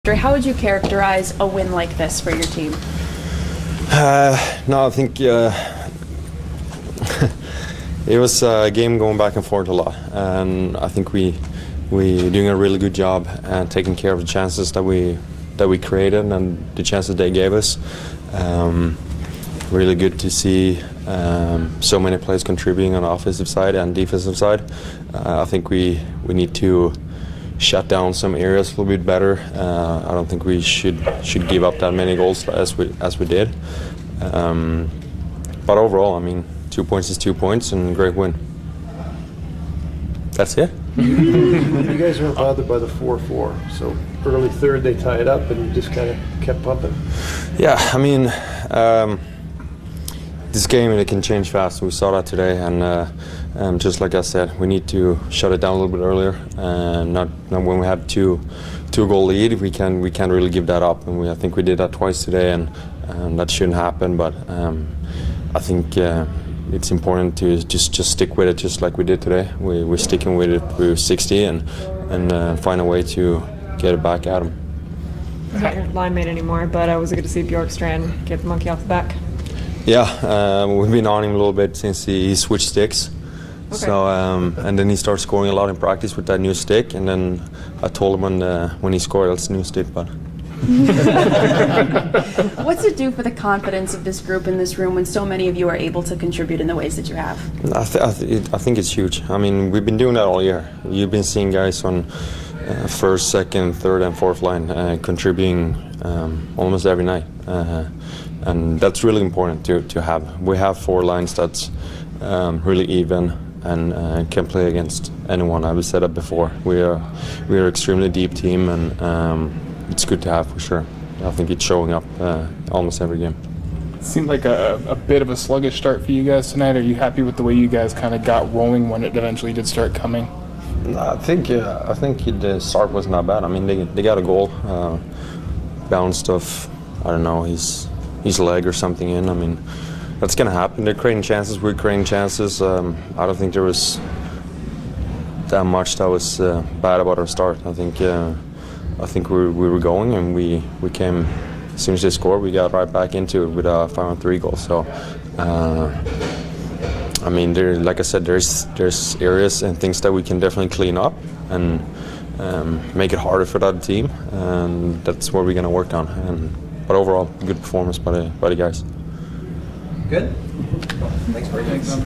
Kraken Sound: Andre Burakovsky - Nov. 23, 2022 Postgame
Seattle Kraken forward Andre Burakovsky talks to the media following a win against the Sharks.